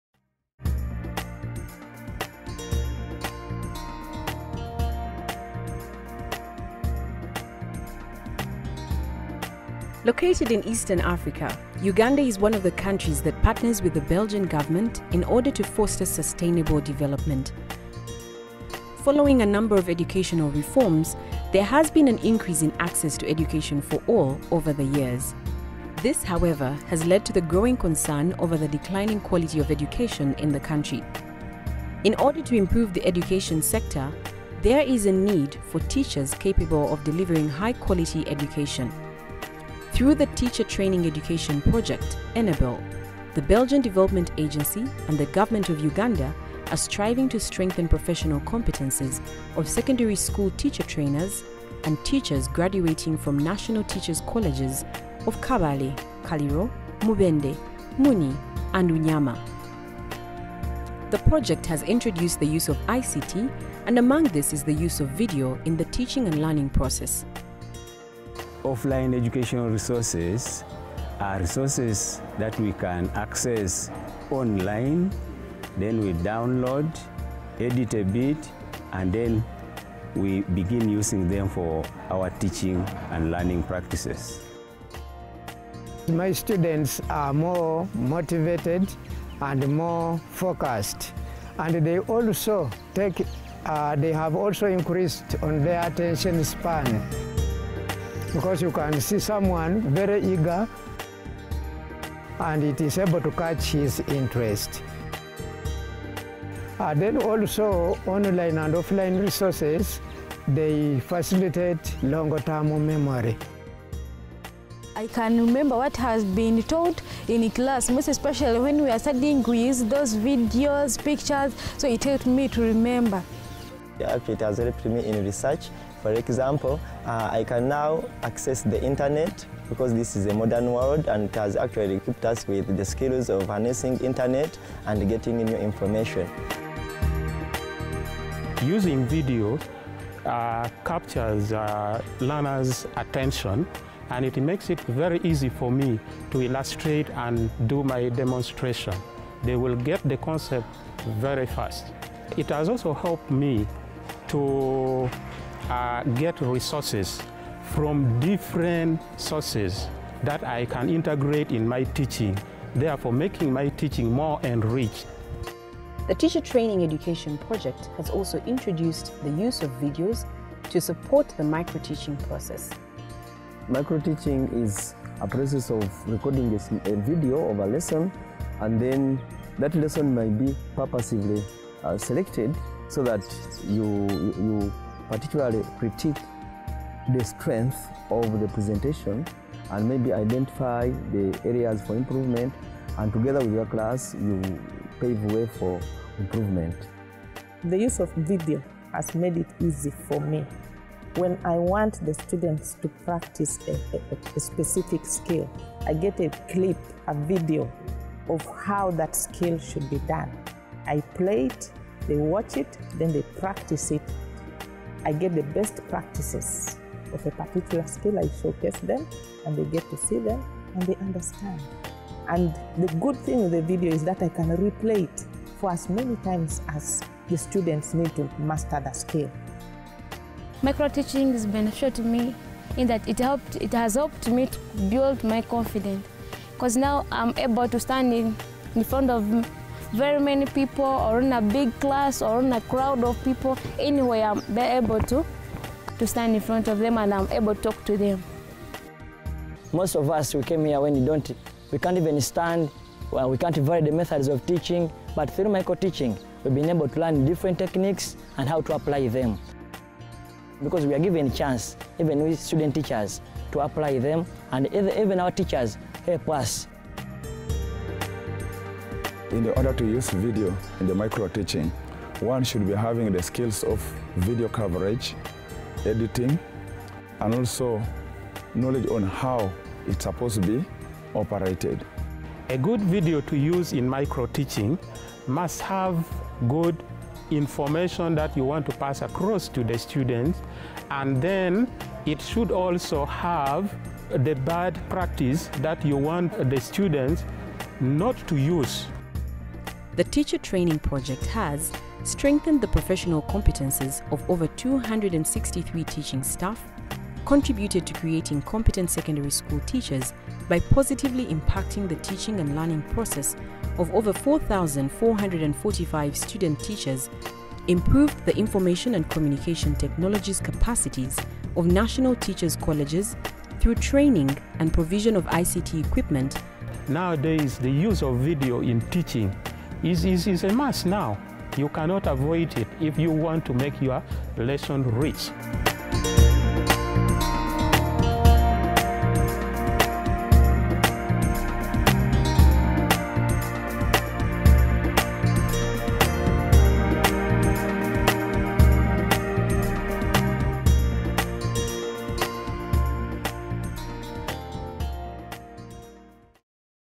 Using videos to improve learning in teacher colleges in Uganda
Quality education relies on the strength of teachers. In Uganda, Enabel supports student-teachers in several national teachers’ colleges. Integrating videos in their classes helps teacher-trainers to improve their lessons and motivate students.